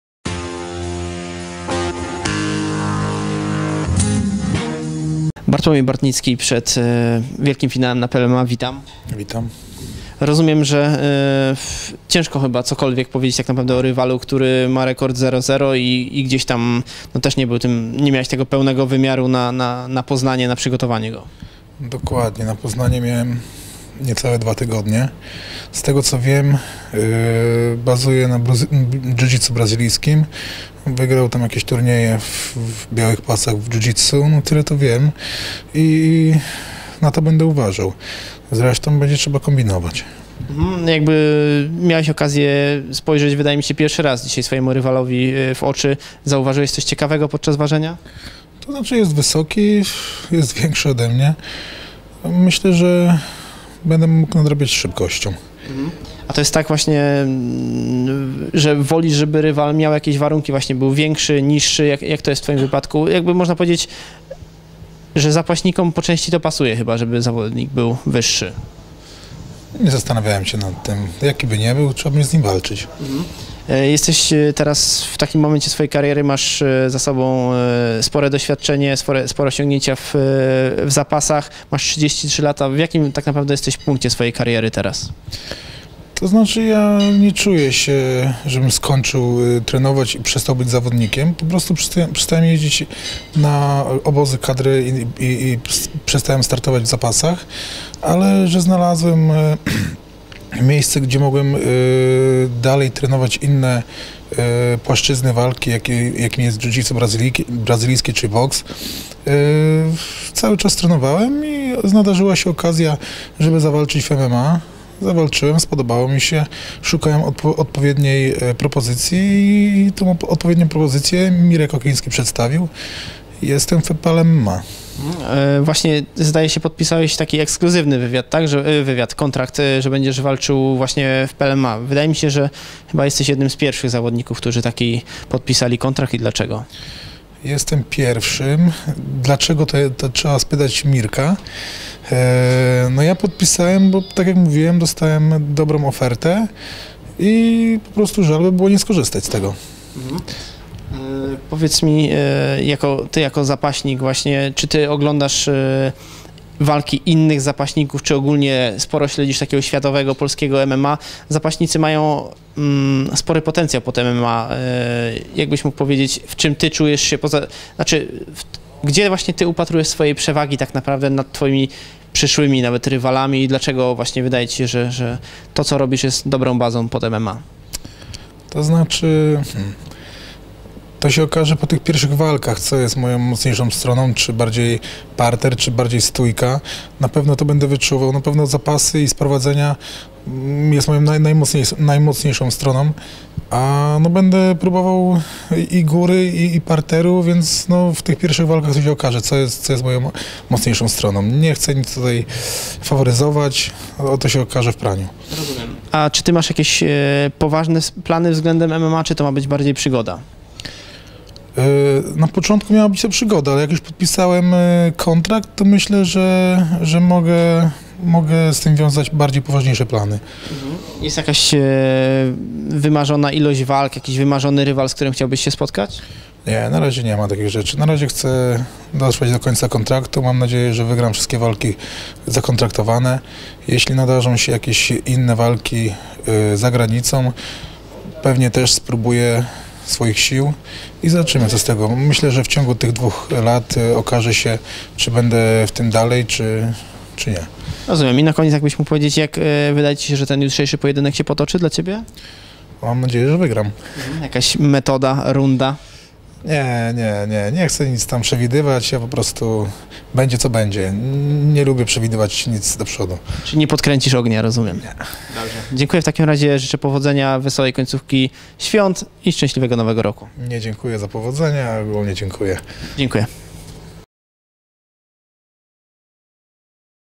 Bartłomiej Bartnicki o podpisaniu ekskluzywnej umowy z PLMMA
Bartłomiej Bartnicki o przygotowaniach przeplatanych świąteczną przerwą i kontrakcie, który podpisał z PLMMA na wyłączność. Poza tym krótko o swoim kolejnym rywalu i kolejnych wyzwaniach w karierze sportowej.